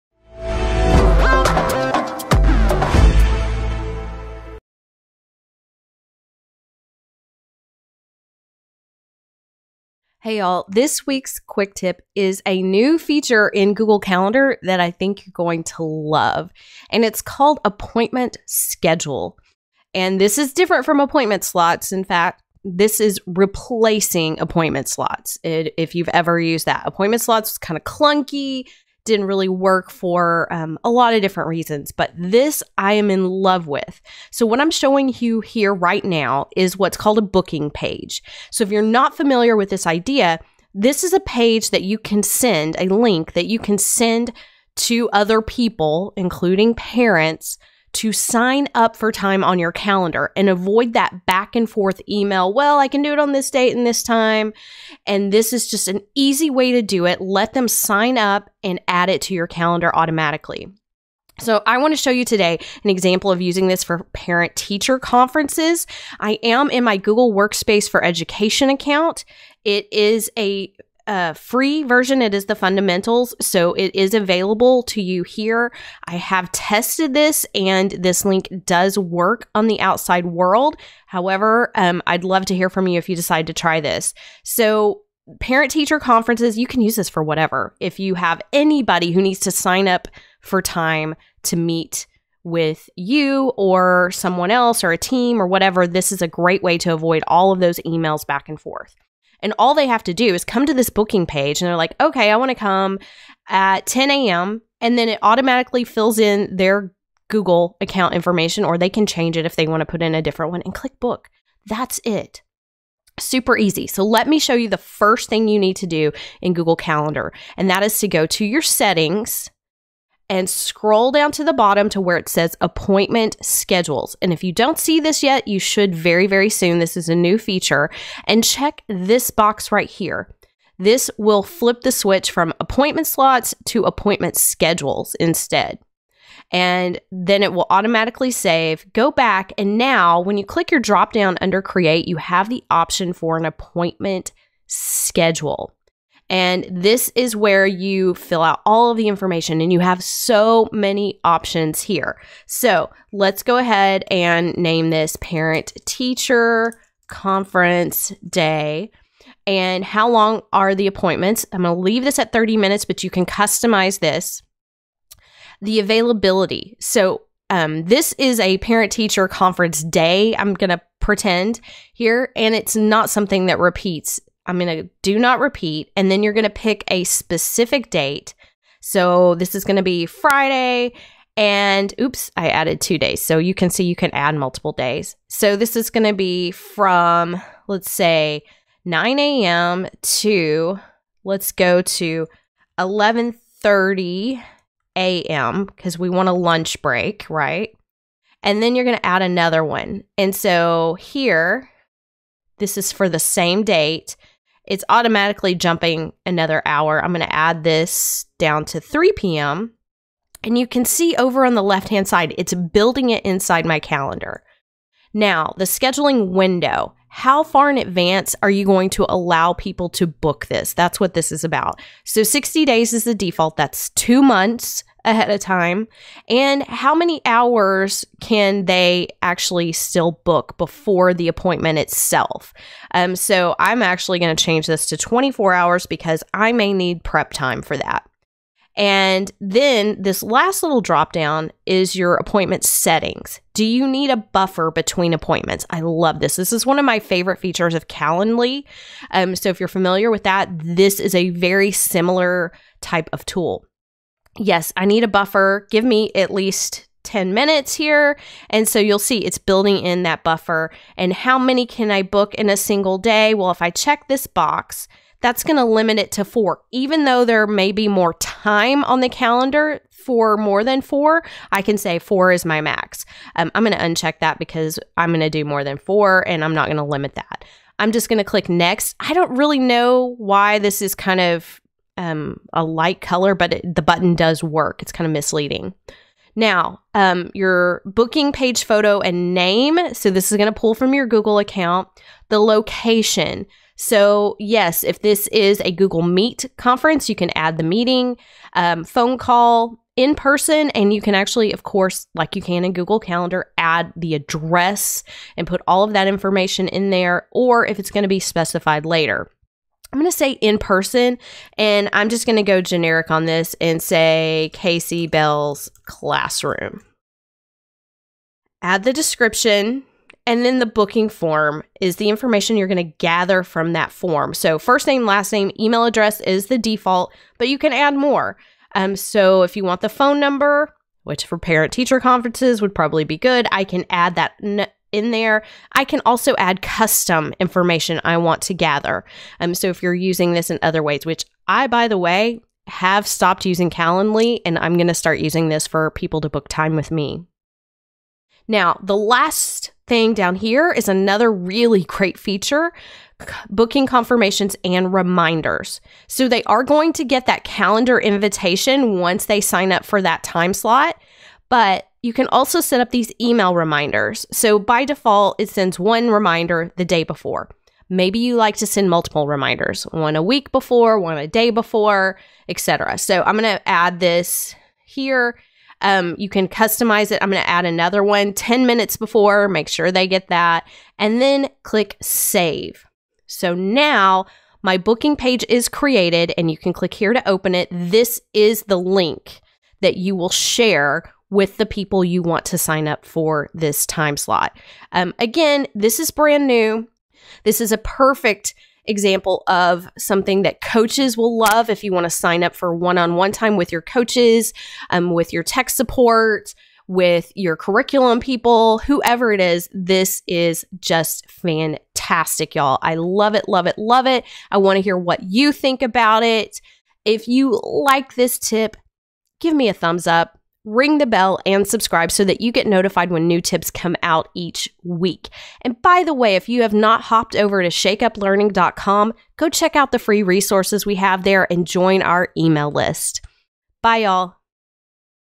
Parent Teacher Conference Sign-Up Appointment Page in Google Calendar!
Google Calendar has an awesome new Appointment Schedule feature that allows you to create a booking page, so parents can sign-up for a time slot for parent teacher conferences!

Use this to book all kinds of appointments! Share a unique booking page with time slots that other people can book! Great for parents, coaching meetings, and more!